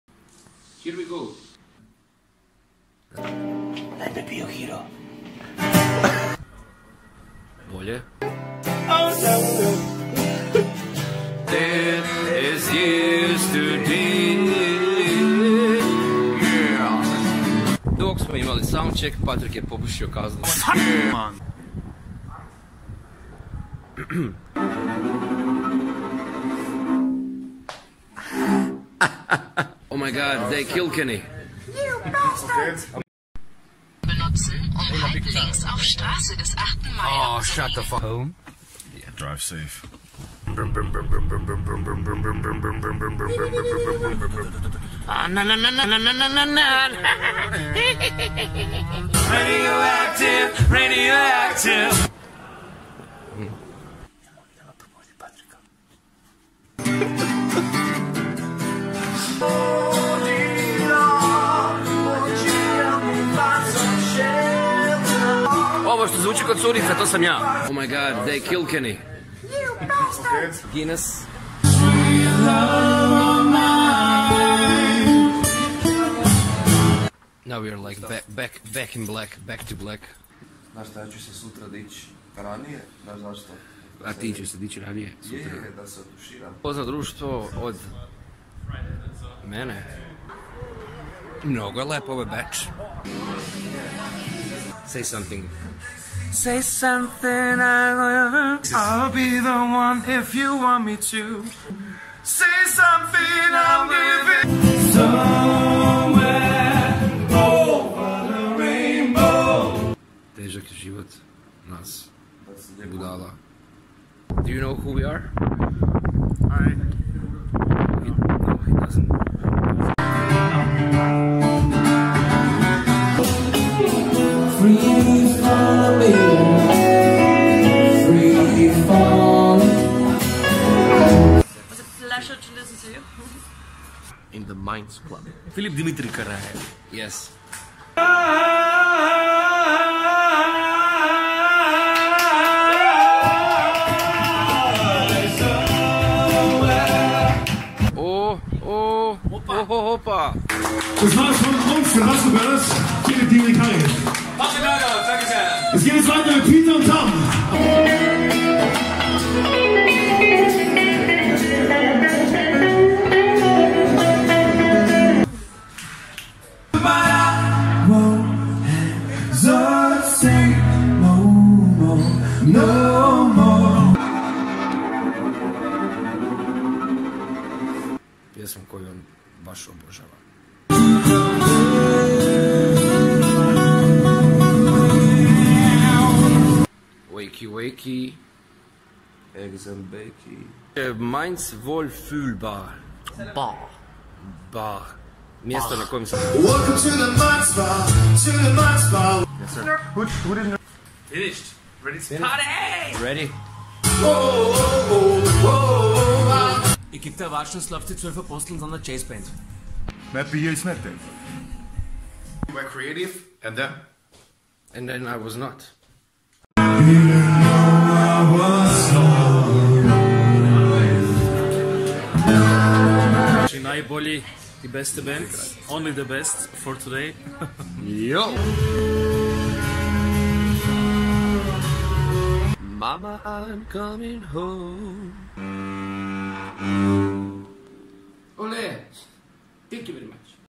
Basco Brothers, Petar Štrok & Tomislav Užarević - Vienna tour Vlog 2019
Recorded by  - Tomislav Užarević, Patrick Neudorf & Petar Štrok
Video editing - Tomislav Užarević & Petar Štrok
Subtitles - Petar Štrok

A fun vlog about the amazing tour we had with the super talented Philipp Dimitri Karajev 'Phil Deekay' & Patrick Neudorf from Basco Brothers acoustic duo. We had such a fun time, and played some amazing shows in awesome places!